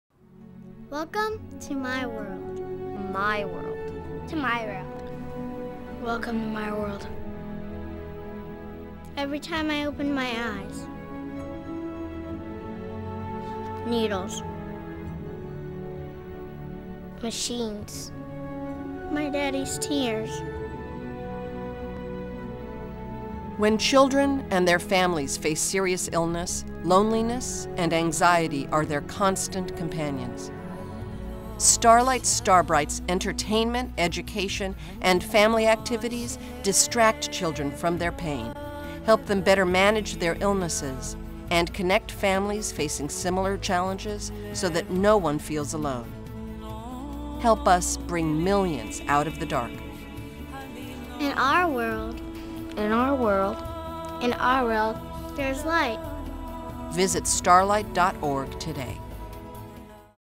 "In My World" -- Starlight Starbright Children's Foundation
Starlight Starbright has completed production on a new television Public Service Announcement entitled "In My World."  The spot features children talking about what living with serious illness is like, and how Starlight Starbright makes a difference.  Jamie Lee Curtis provides the spot's narration. to find out how you can help.